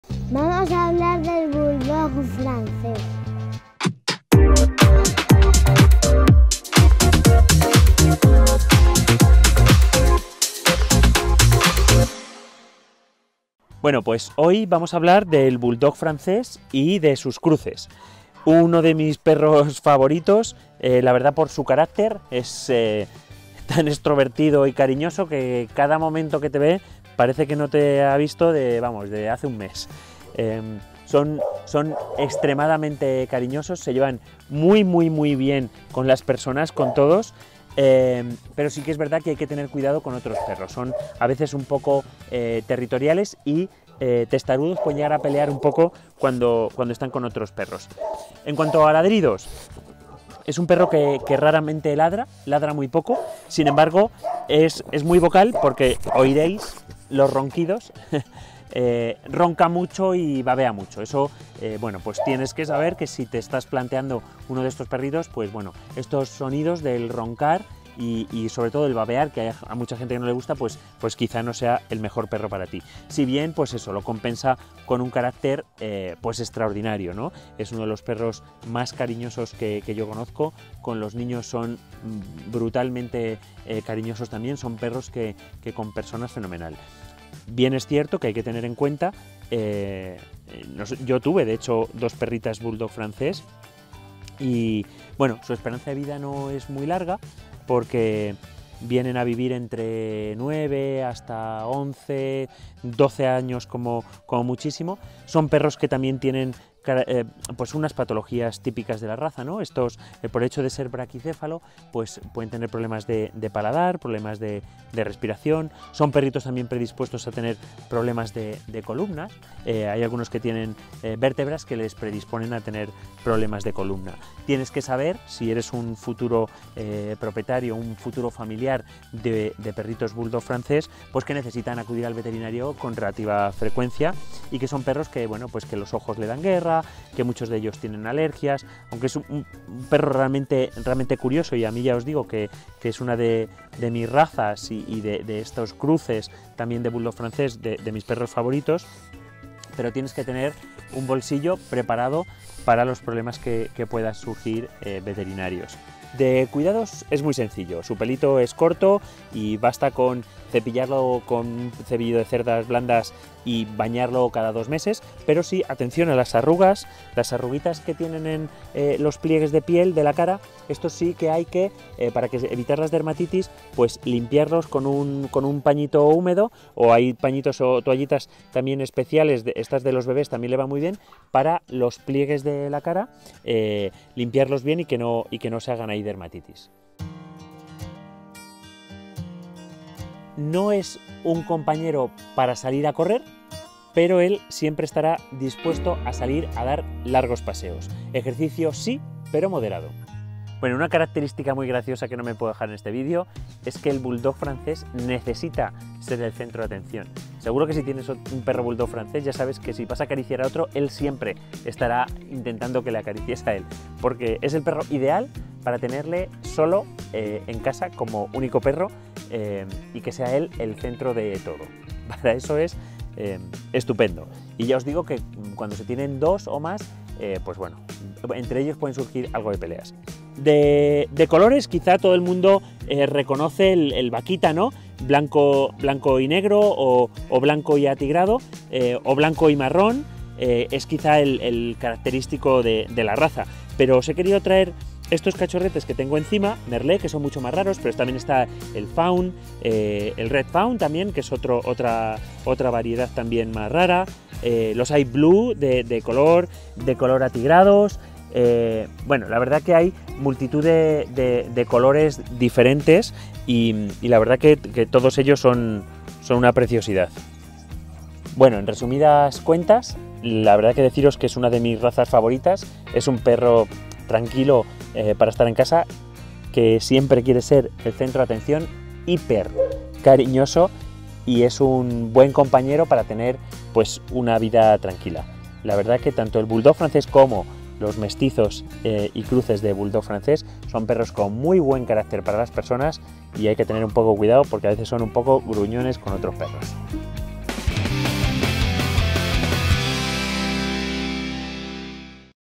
🐕Descubre TODO lo que debes saber sobre el Bulldog Francés*
Nuestro veterinario Antonio Rodríguez nos explica con pelos y señales TODO lo que debemos saber sobre el Bulldog Francés. Si estás pensando en acoger a uno no te puedes perder este vídeo, en él podrás saber los problemas veterinarios más frecuentes, detalles sobre su morfología e incluso la variedad de colores entre otras cosas.

También puedes seguirnos en Facebook, Twitter o Instagram (te responderemos al momento 😉)